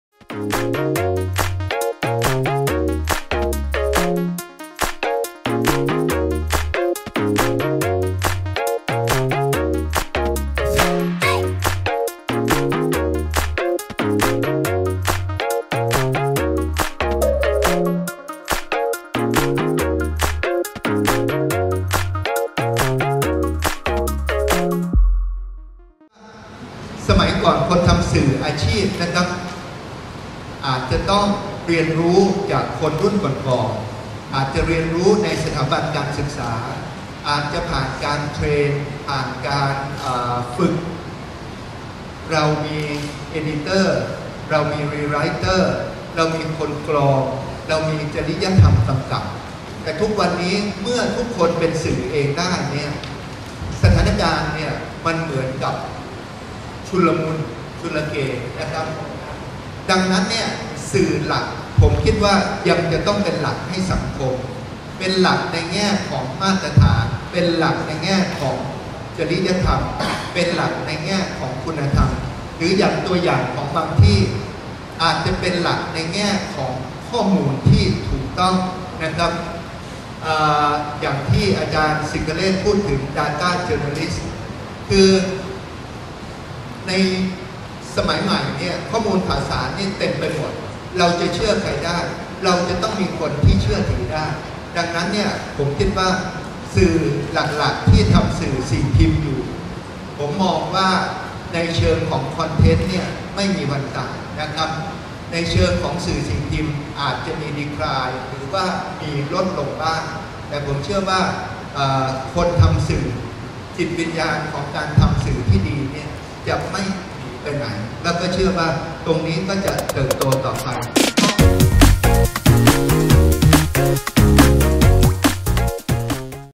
“กองทุนพัฒนาสื่อปลอดภัยและสร้างสรรค์” กับความร่วมมือในการขับเคลื่อนสื่อปลอดภัยและสร้างสรรค์
สิ่งพิมพ์: 'วิกฤต' คือ 'โอกาส' และบทบาทของกองทุนพัฒนาสื่อปลอดภัยและสร้างสรรค์  วันที่ 23 พฤศจิกายน 2559  ณ KX: Knowledge Xchange

โดย วสันต์ ภัยหลีกลี้ ผู้จัดการกองทุนพัฒนาสื่อปลอดภัยและสร้างสรรค์
ติดตาม “กองทุนพัฒนาสื่อปลอดภัยและสร้างสรรค์” ในช่องทางอื่นๆ ได้ที่